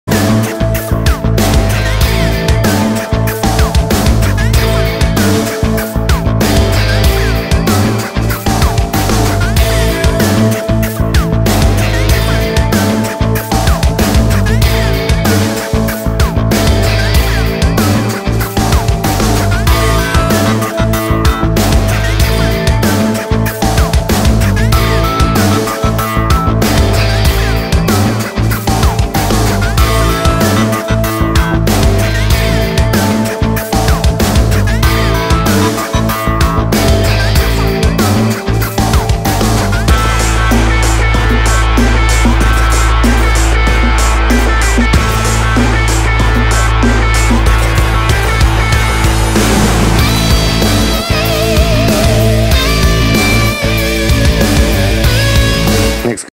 Ilya Lukavets, Belarus, 100 kg
3 место в категории до 100 кг, "Мистер Олимпия Москва" - Илья Луковец (Беларусь), произвольная программа